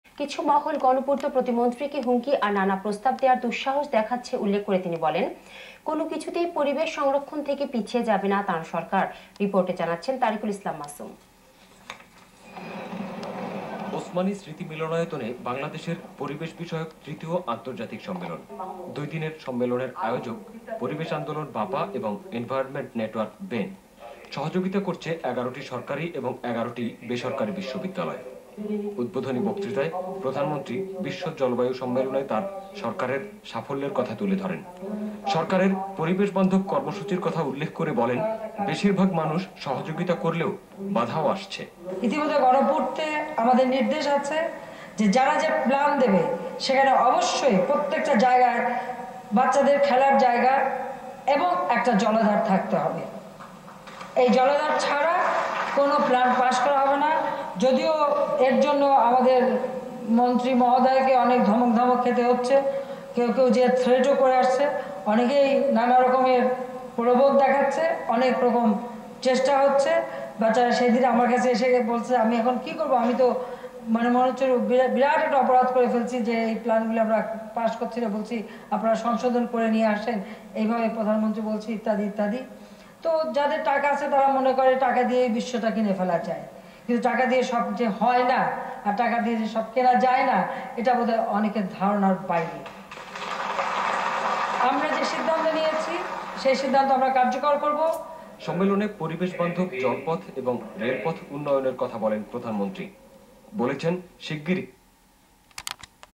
Prime Minister Sheikh Hasina speaks at BEN-BAPA Conference
Honarable Prime Minister Sheikh Hasina speaks at the ICBEN 2010 Conference held in Dhaka on January 3-4, 2010, organized by BEN and BAPA.  The conference was attended by many dignataries including Dr. Hasan Mahmud (State Minister), Nazrul Islam (UGC Chair), AAMS Arefin Siddique (VC DU), Prof. Muzaffar Ahmed (President BAPA), Dr. Nazrul Islam (BEN Coordinator), and Dr. A. Matin GS BAPA)